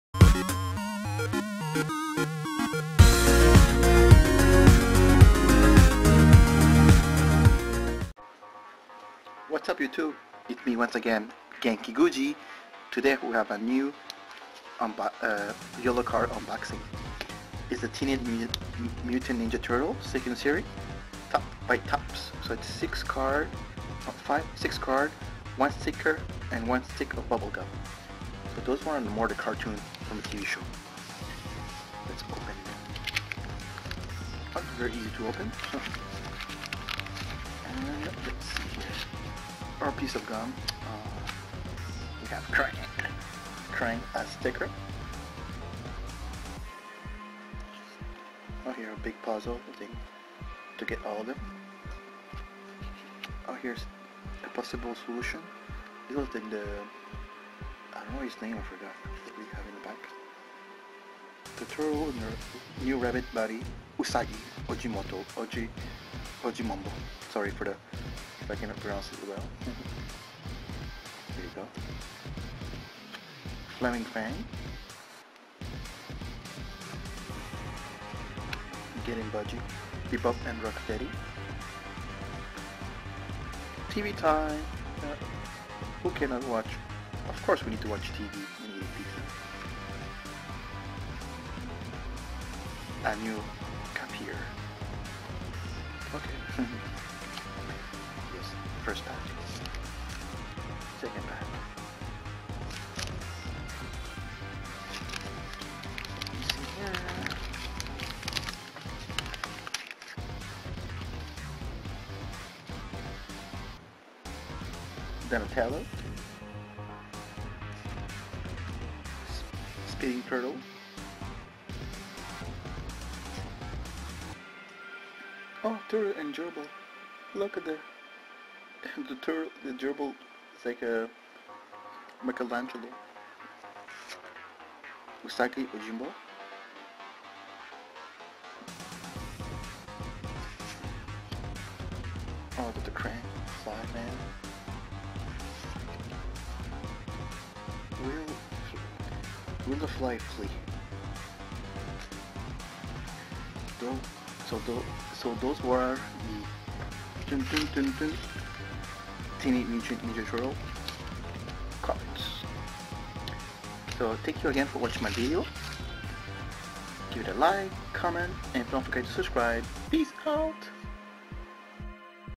1990 Topps TEENAGE MUTANT NINJA TURTLES WAX PACK
1990 Topps : Teenage Mutant ninja turtle the carton show
Every one love those funny turtle ! All 90s kids love eating pizza and wanted to learn kunfu
Join my adventure opening some old memories and maybe getting flashback:p
Peace out